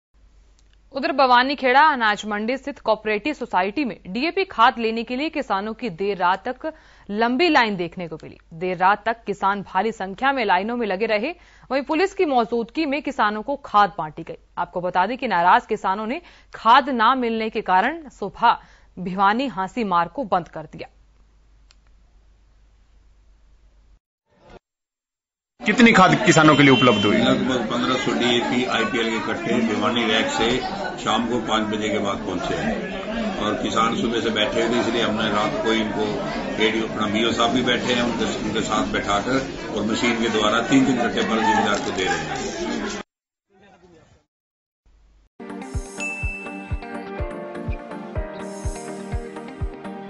किसानों को देर रात मिली खाद, पुलिस की मौजूदगी में बांटी खाद || STV Haryana News
बवानीखेड़ा अनाज मंडी स्थित कोऑपरेटिव सोसाईटी में डीएपी खाद लेने के लिए किसानों की देर रात तक लंबी लाइन देखने को मिली... देर रात तक किसान भारी संख्या में लाइनों में लगे रहे। वहीं पुलिस की मौजूदगी में  किसानों को खाद बांटी गई।